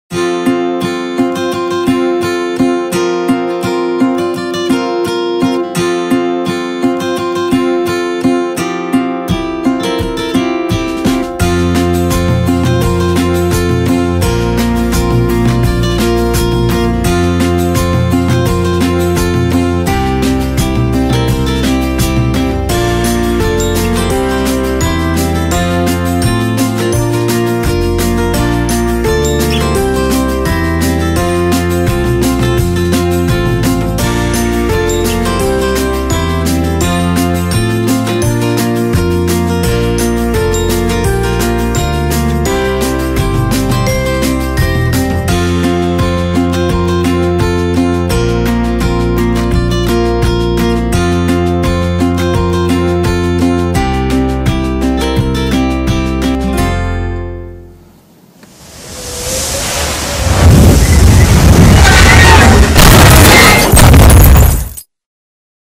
Travelling to Bergen with a very dramatic ending...
Going to demonstrate the complete democratization of production and distribution in a presentation tomorrow with this quick video that I made on my iPhone on my way to the venue...